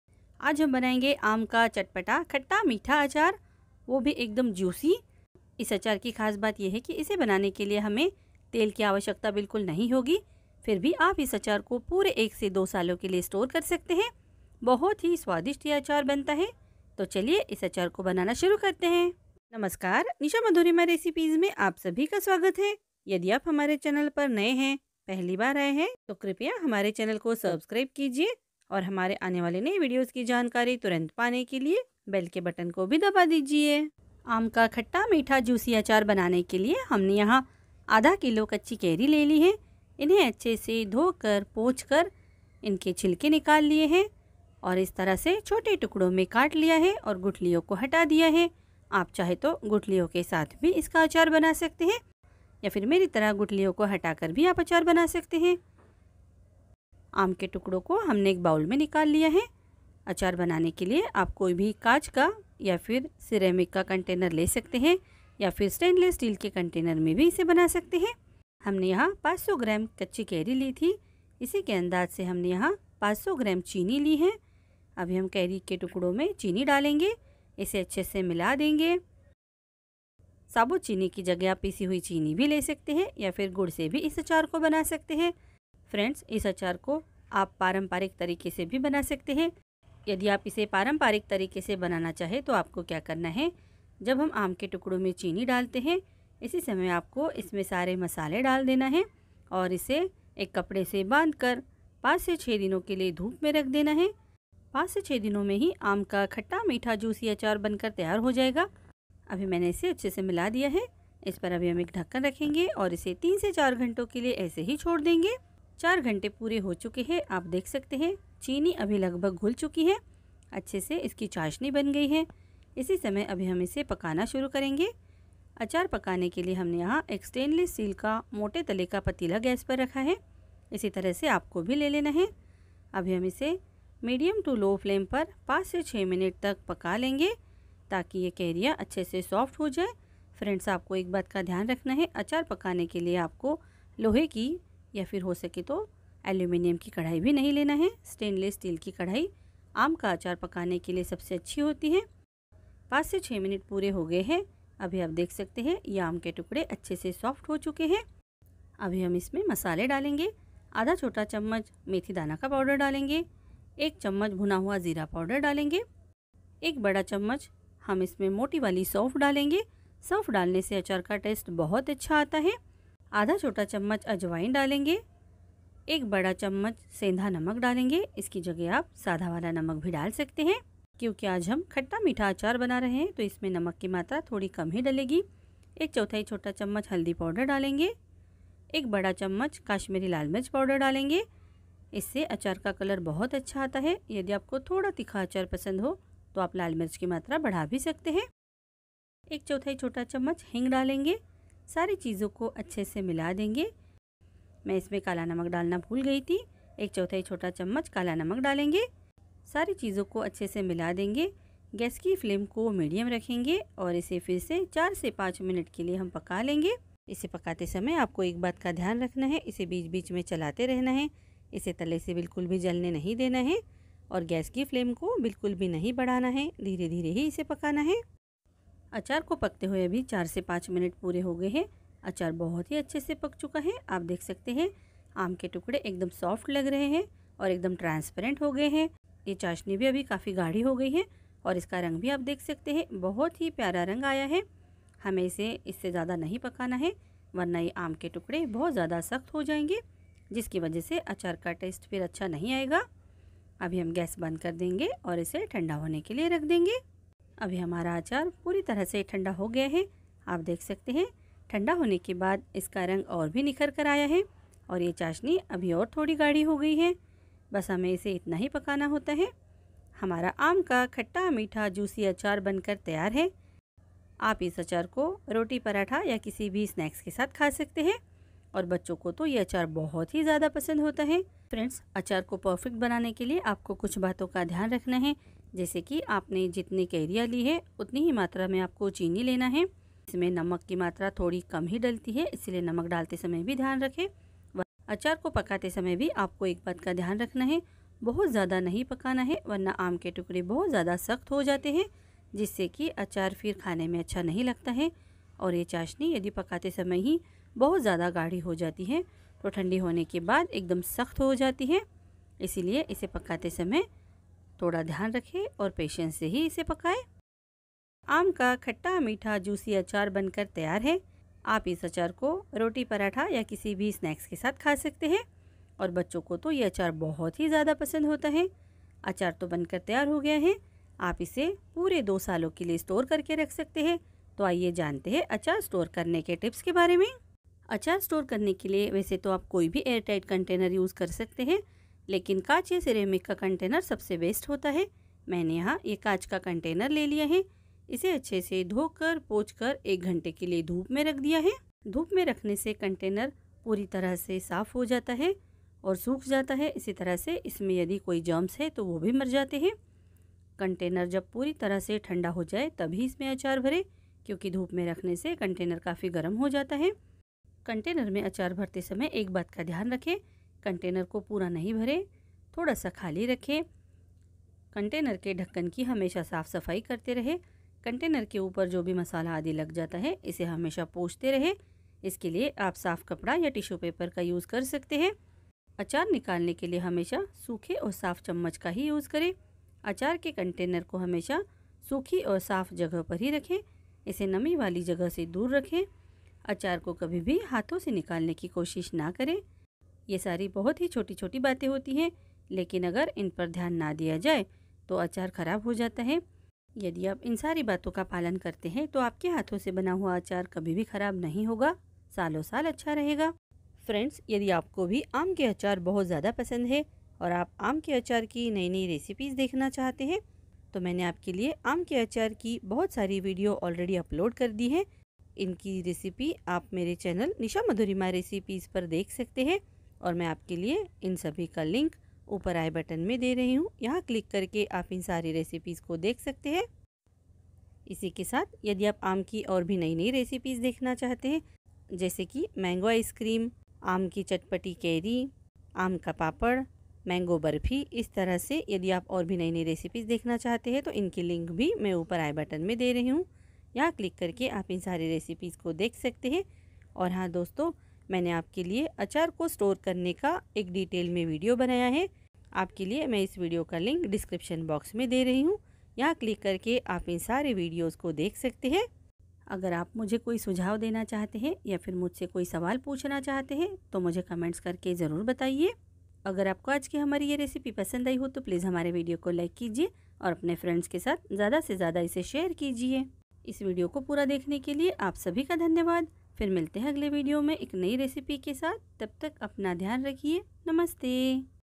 आम का खट्टा मीठा अचार ऐसे बनाए 1 साल आसानी से स्टोर करे वो भी बिना तेल के|Aam Ka Meetha Achar|Mango
Ingredients
Raw mangoes 500g
Sugar 500g
Rock salt 1 tbsp
Fenugreek seed powder 1/2 tsp
Fennel seeds 1 tbsp
Ajwin 1/2tsp
cumin powder 1 tsp
सामग्री
कच्चा आम 500 ग्राम
चीनी 500 ग्राम
लाल मिर्च पाउडर 1 बड़ा चम्मच
हल्दी 1/4 छोटा चम्मच
सेंधा नमक 1 बड़ा चम्मच
काला नमक 1/2 छोटा चम्मच
हींग 1/4 छोटा चम्मच
मेथी दाना पाउडर 1/2 छोटा चम्मच
सौंफ के बीज 1 बड़ा चम्मच
अज्विन 1/2 छोटा चम्मच
जीरा पाउडर 1 छोटा चम्मच